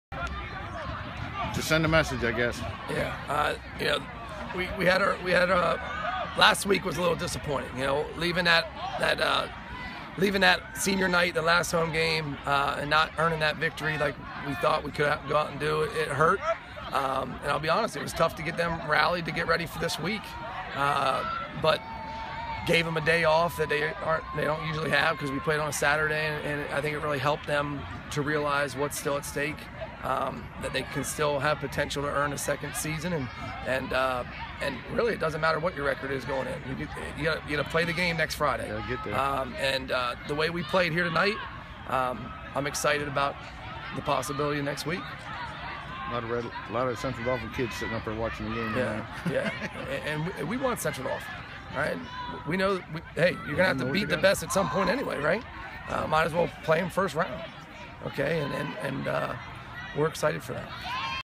Carlisle football coach Josh Oswalt on his team looking forward to facing Central Dauphin in D3-AAAA
The District 3-AAAA bracket is still unofficial but it appears Carlisle is headed for a week 11 showdown with top-seed Central Dauphin.